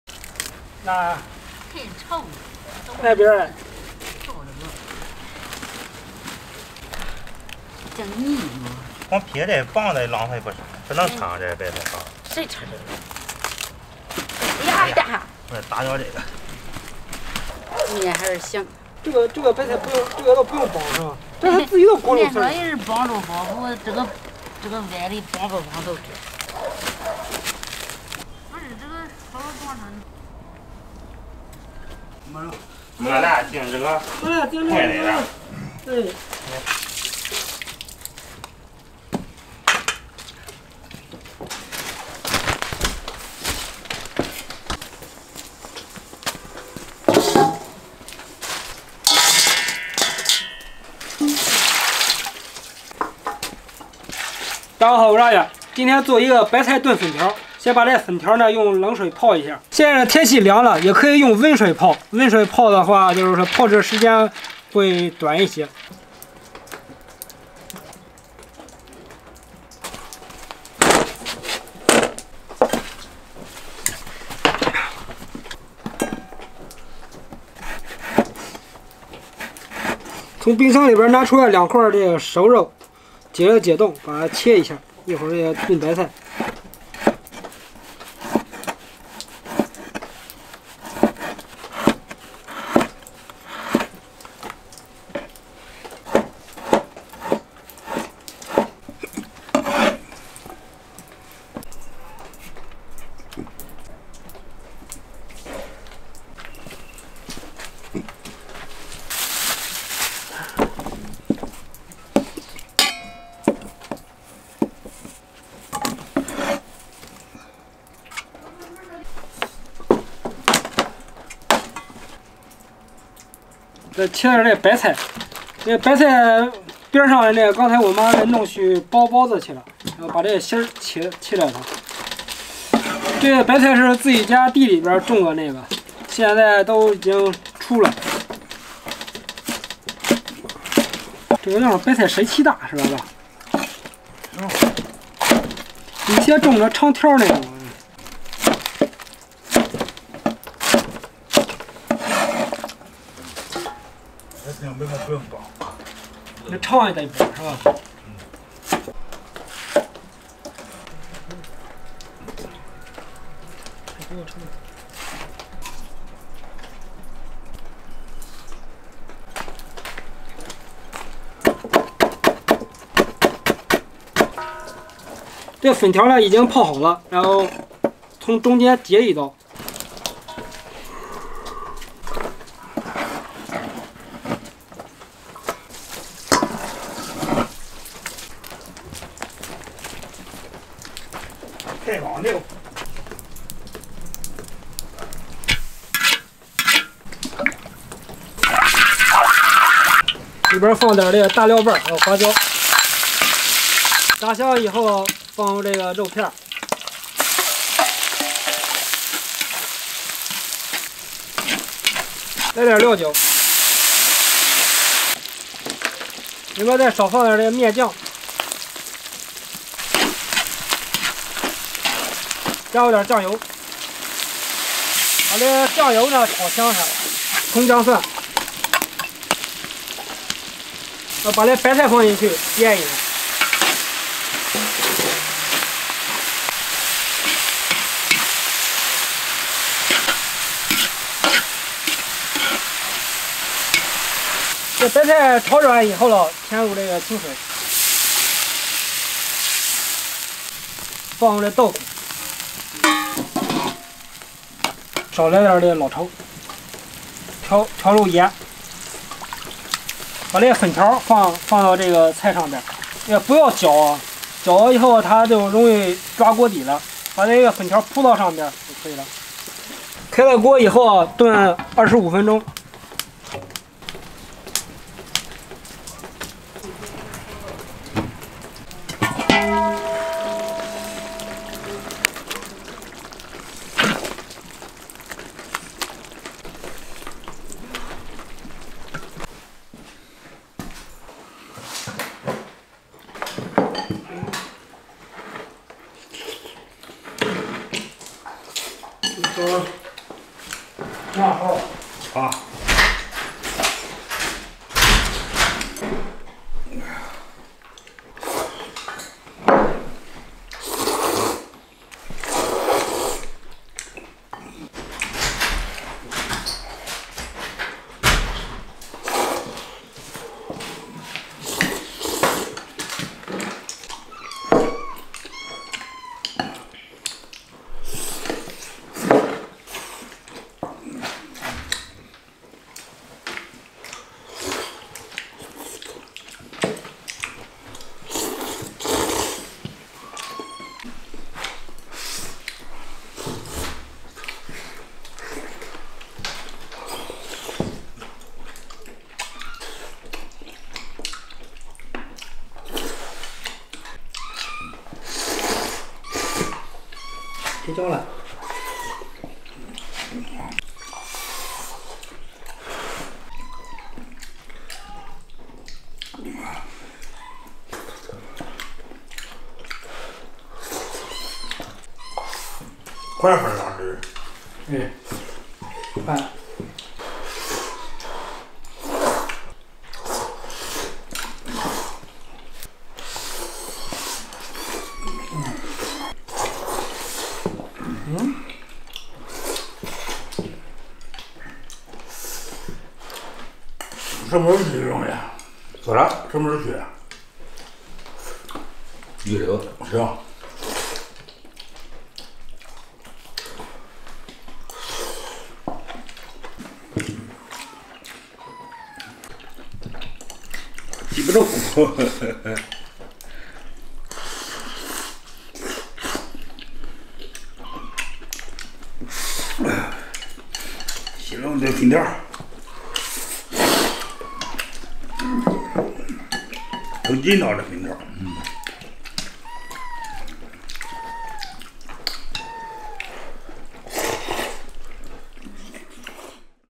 【食味阿远】阿远做猪肉白菜炖粉条，冷天咝溜粉条吃，大伯和老爸都说真地道 | Pork and Cabbage Stewed Vermicelli | Shi Wei A Yuan
大家好，我是阿远，今天做猪肉白菜炖粉条吃

【食味阿远】三农达人
专注原创，分享美味，好吃的原创美食
跟大家一起享受美食带来的快乐！